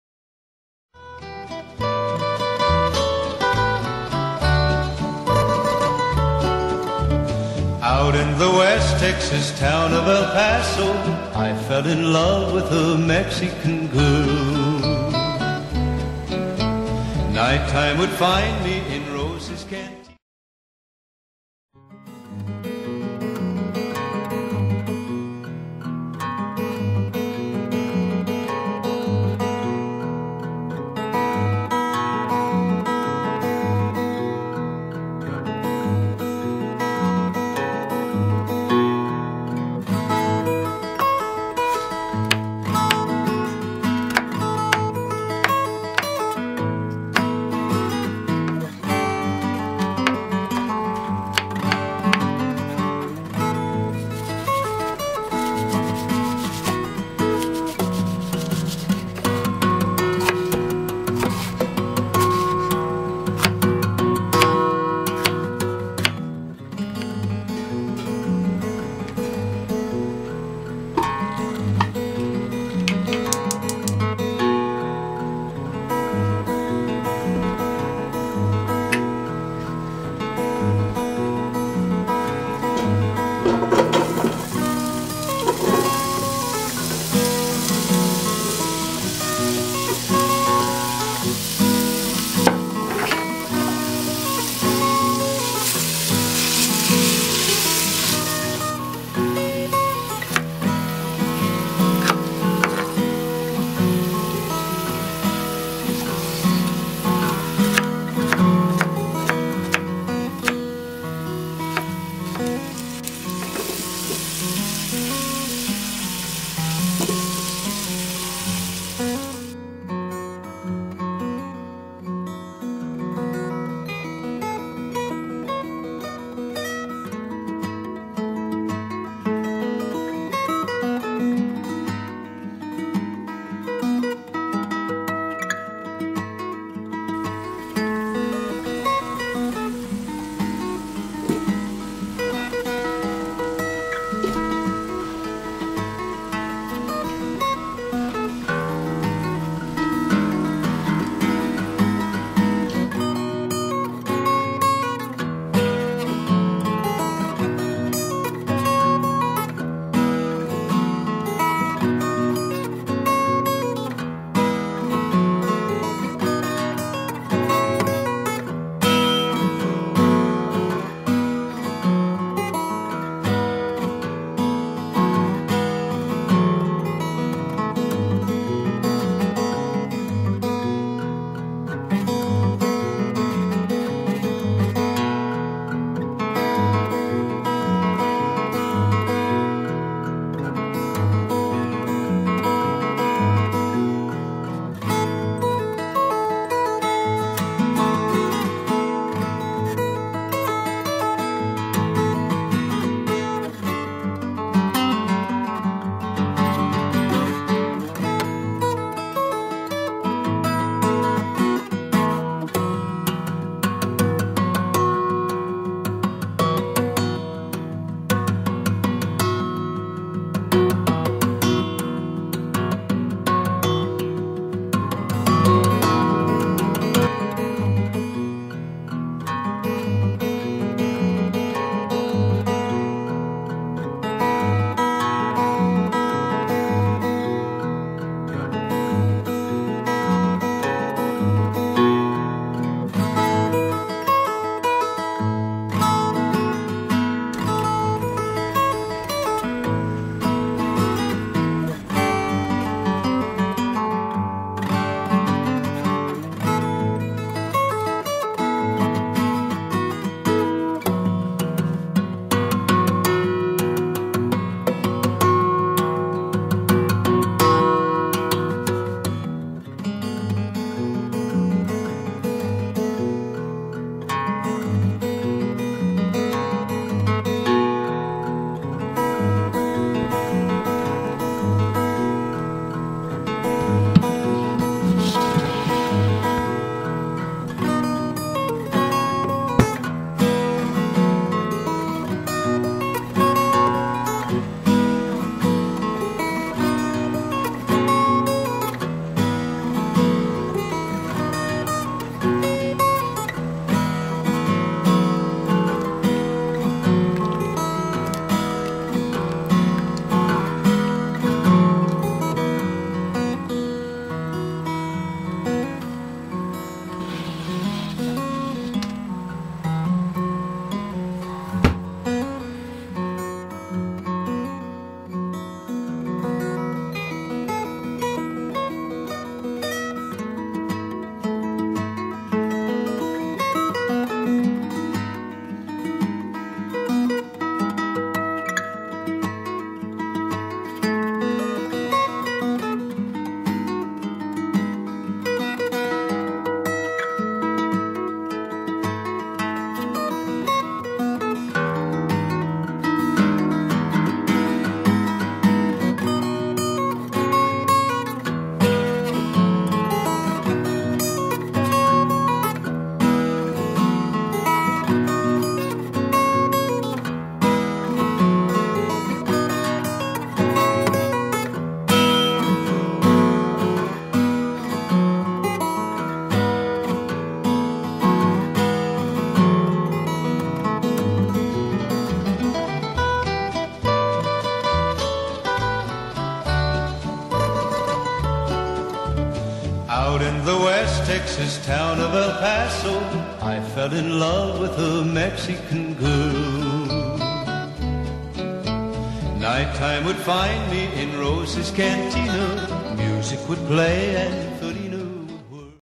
mesquite smoked beef brisket chili
When you have a little brisket left over after a barbecue, why not turn it into a smoky and tasty chili.
  The fire roasted tomatoes along with the smoked beef brisket give this chili a wonder smoky flavor that makes this one unique